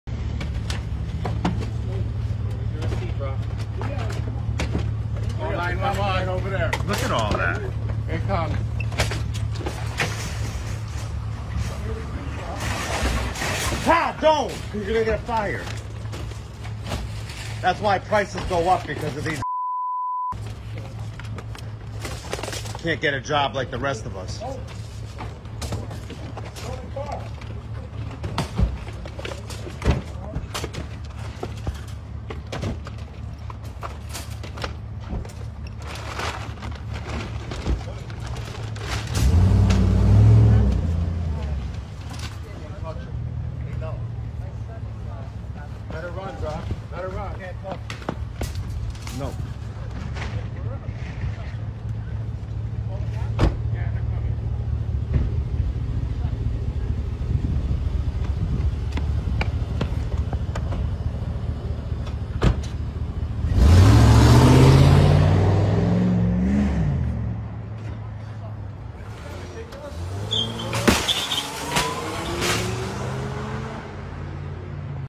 CAUGHT ON CAMERA: Suspects steal grocery carts full of items in Oxford
An eyewitness captured multiple suspects stealing shopping carts full of items from an Oxford grocery store early Tuesday morning.